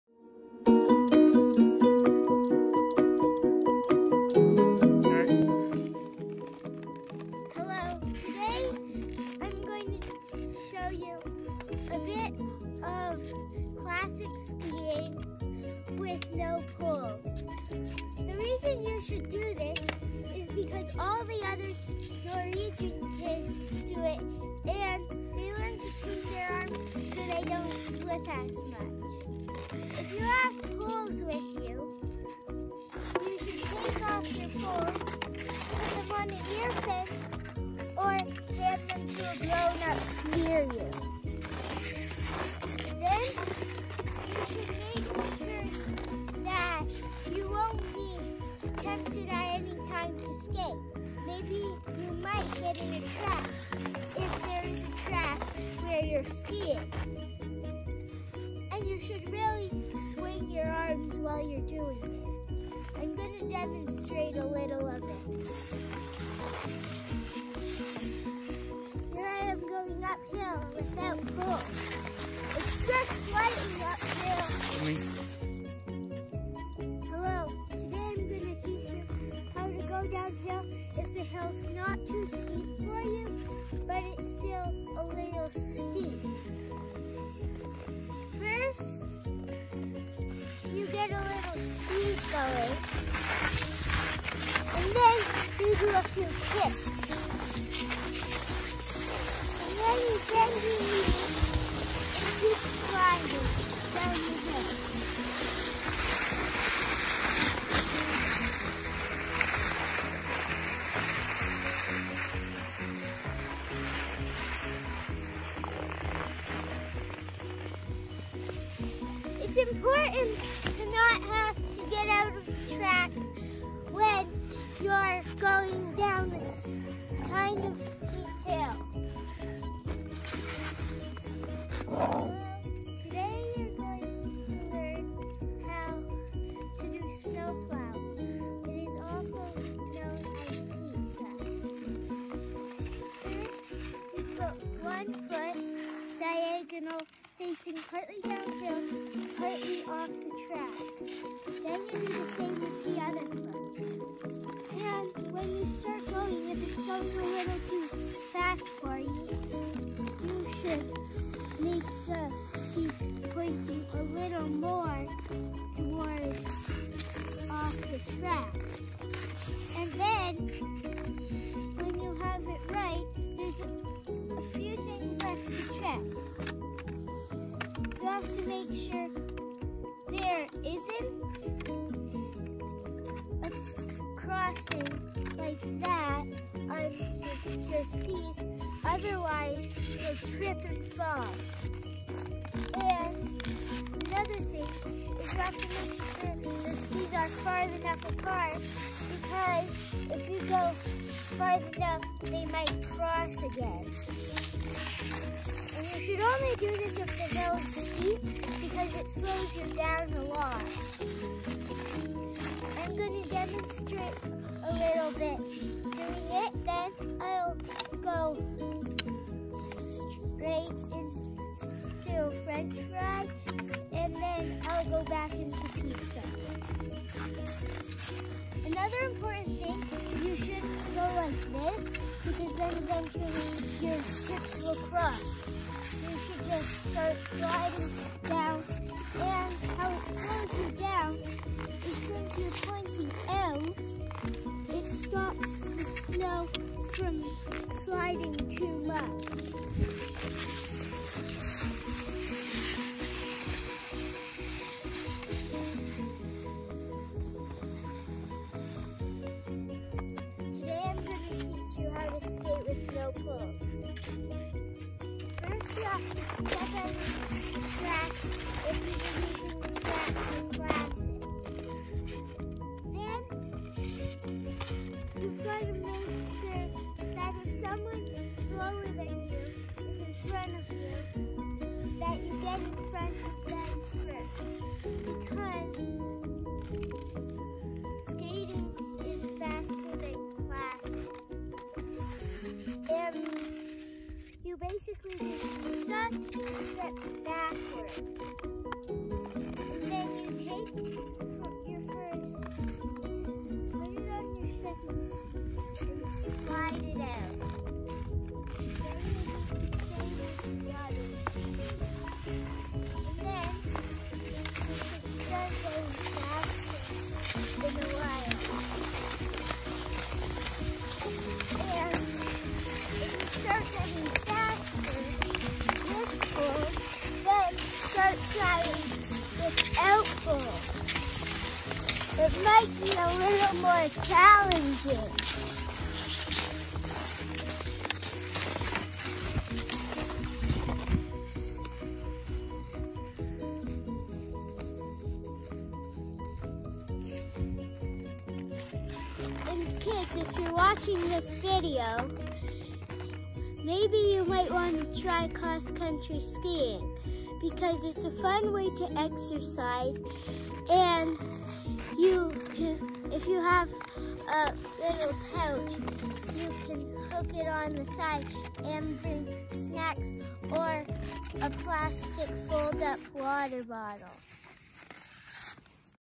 Cross Country Skiing Lessons for Kids
Basic how to instructions filmed at Fraser Colorado.  A Labrador Retriever joins the fun.